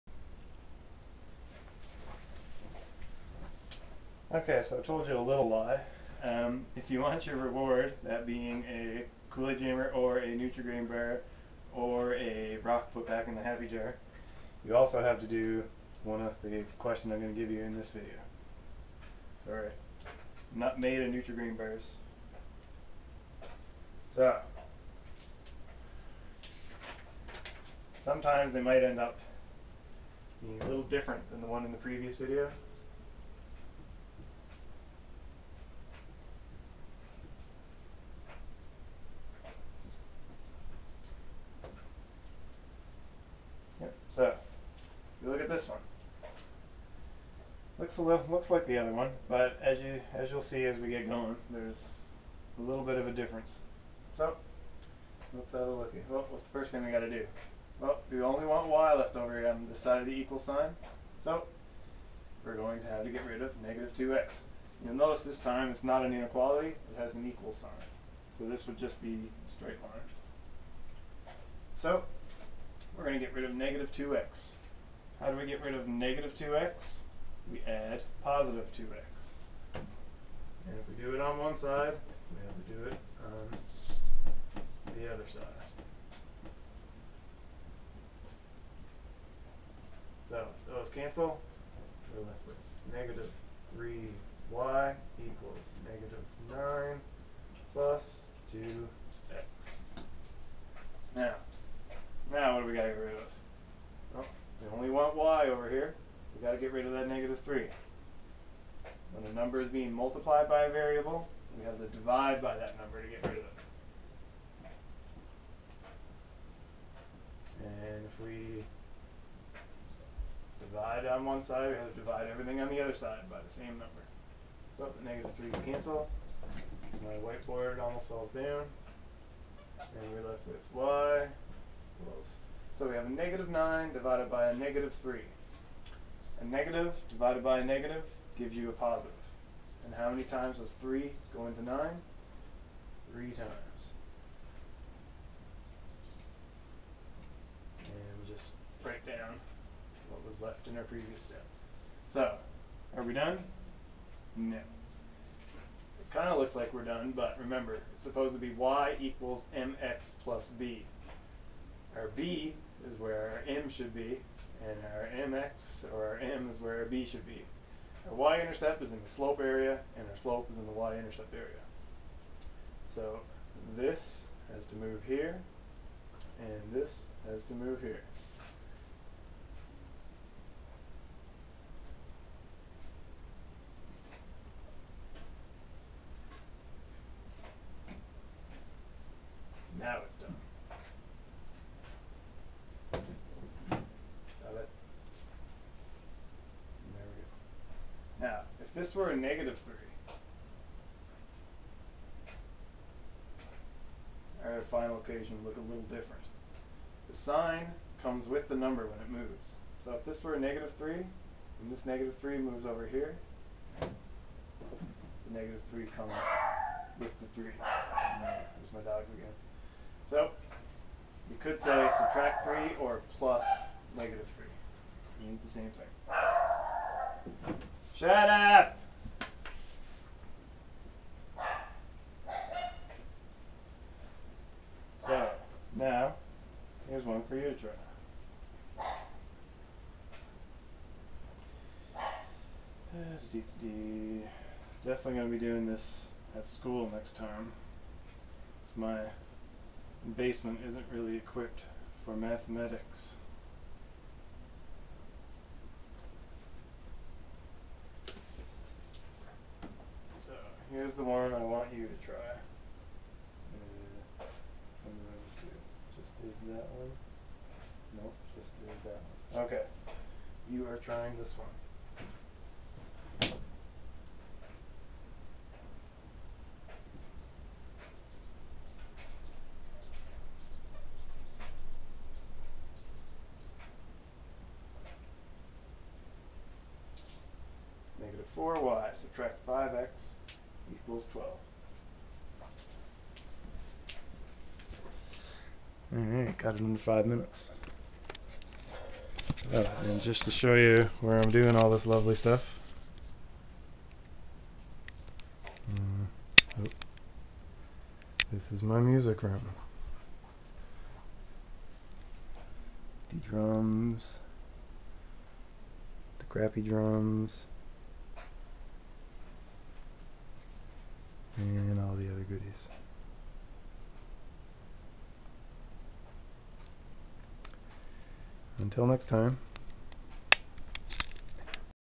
rearranging equations in y=mx+b form 2.MOV
This is another example of an equation that you have to change into y=mx+b format.